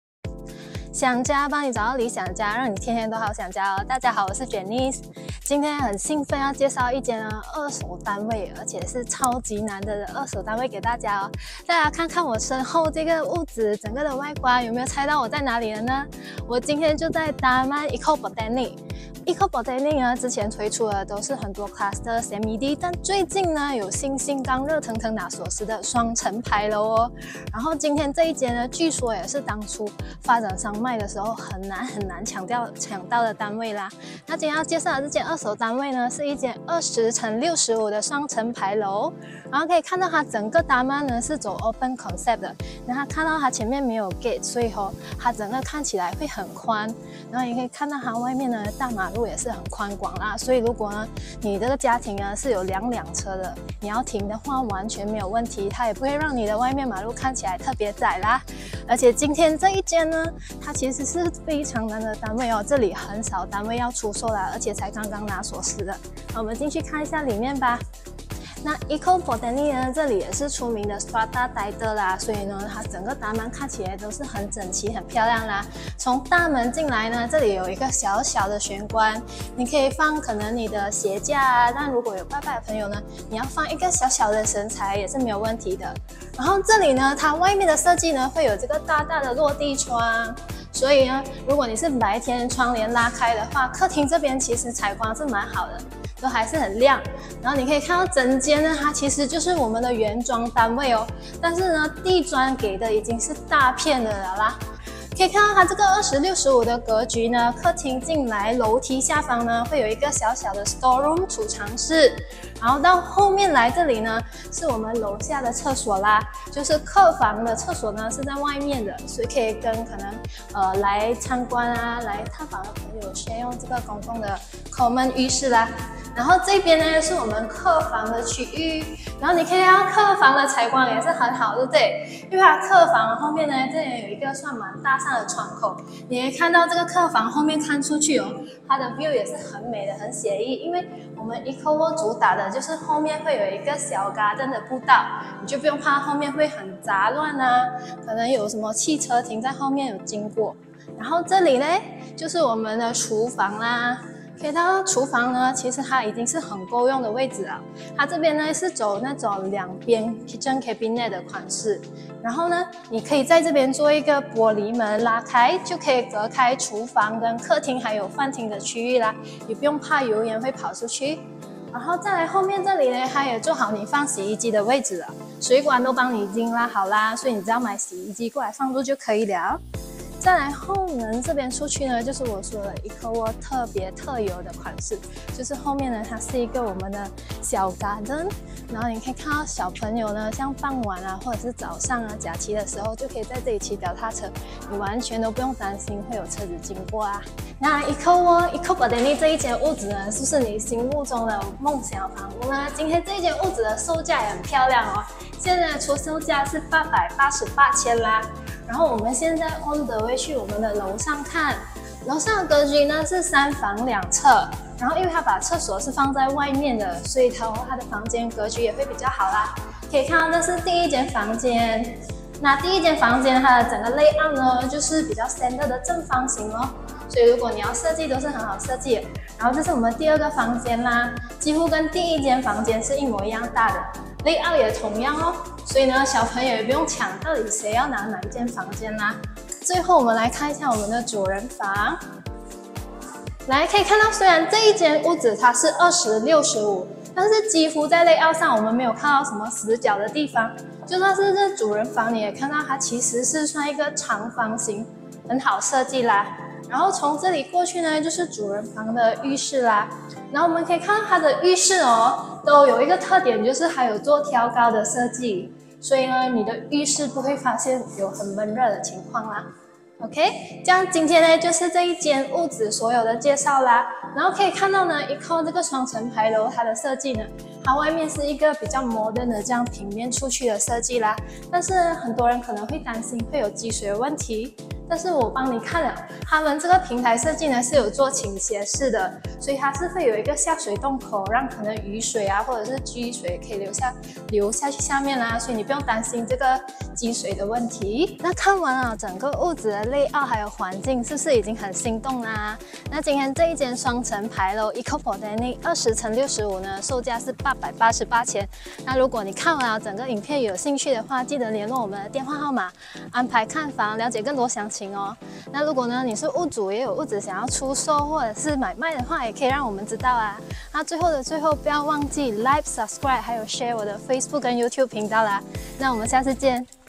【JB Property 新山房地产】Eco Botanic最新热腾腾的双层排楼🔥🔥超美的环境才刚刚拿到钥匙的就已经有很多人在找的热门地点😍带你一起来看看这个地方有什么吸引人的吧‼️
Eco Botanic最新热腾腾的双层排楼🔥🔥超美的环境才刚刚拿到钥匙的就已经有很多人在找的热门地点😍带你一起来看看这个地方有什么吸引人的吧‼️